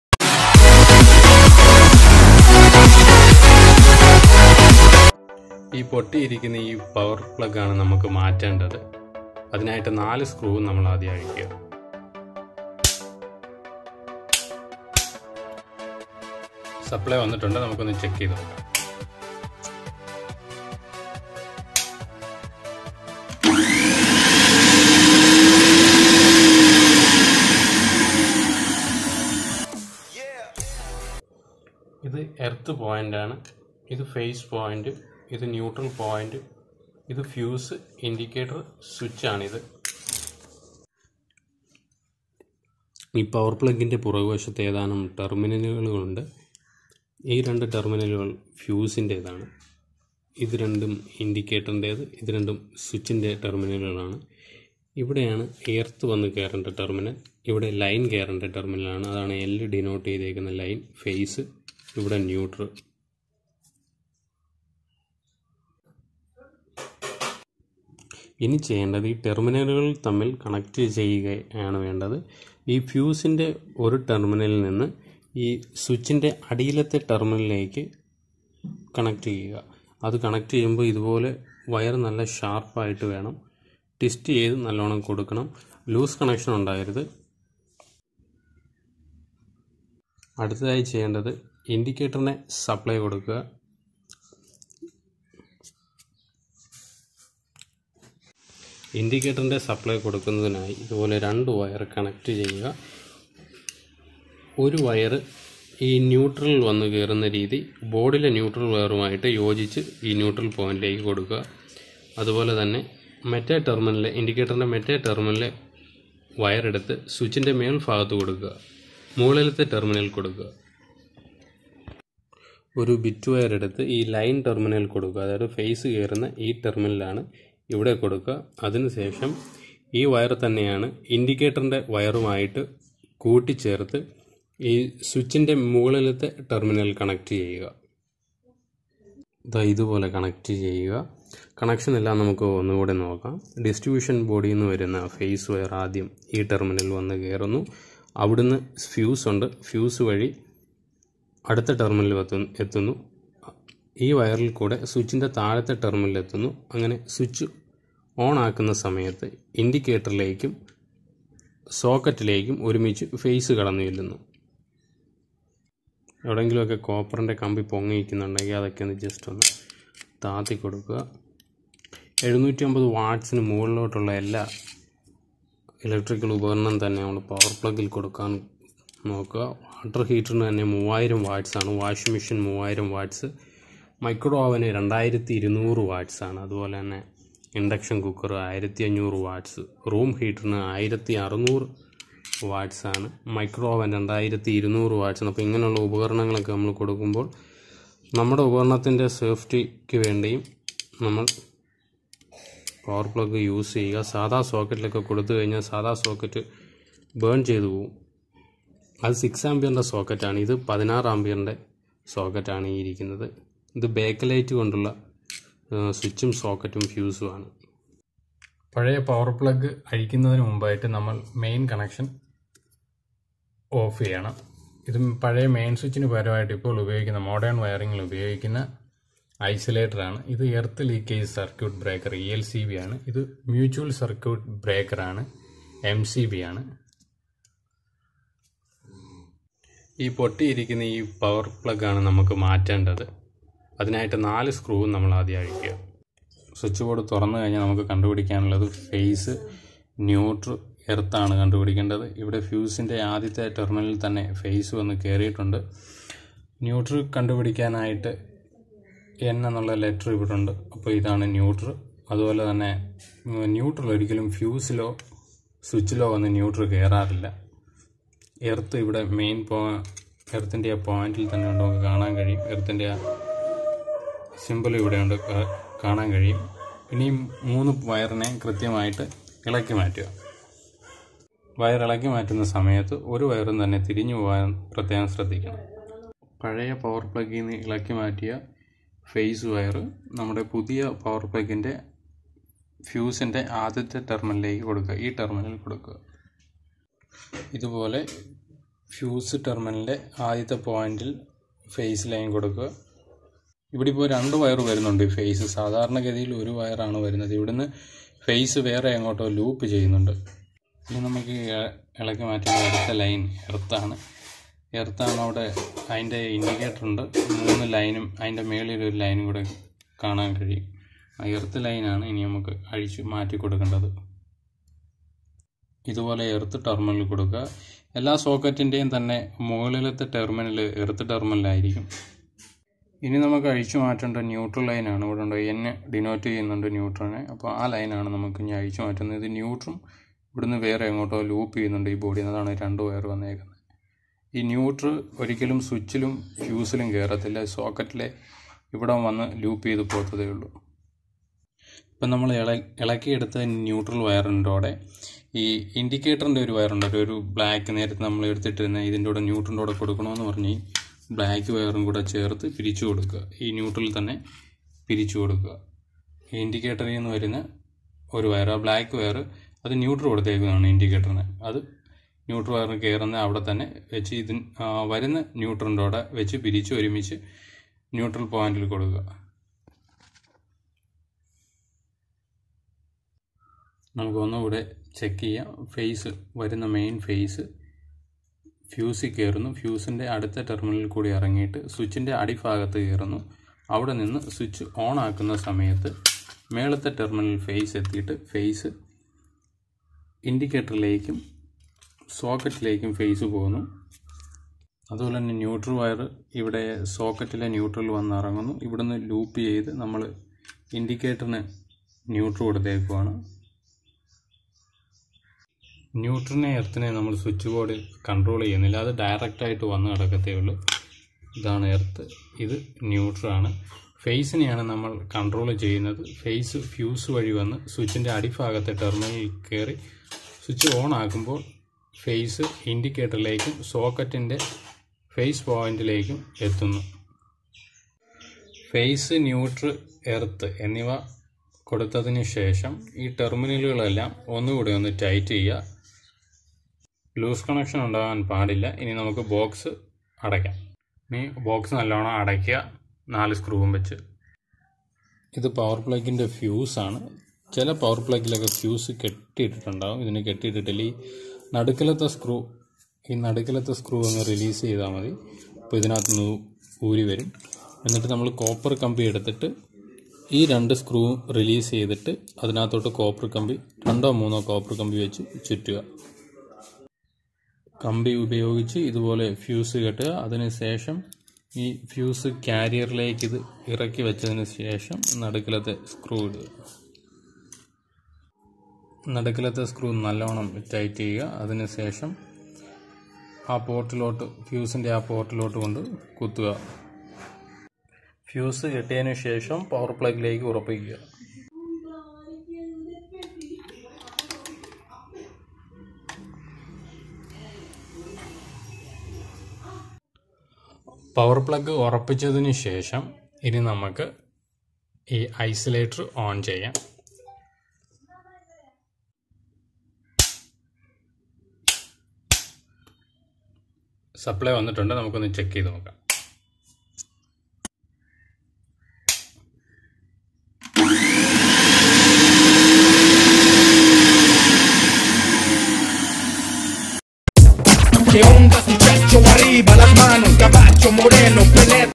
വെറും 5 മിനിറ്റ് കൊണ്ട് പവർ പ്ലഗ് എങ്ങനെ ഫിറ്റ് ചെയ്യാം!
എന്തിനാണ് പവർ പ്ലഗ് ഉപയോഗിക്കുന്നത് ?

Power plug
# single phase
# Earth Pin
washing machines
# bestmicrowave
# Bestwaterheater
# MCB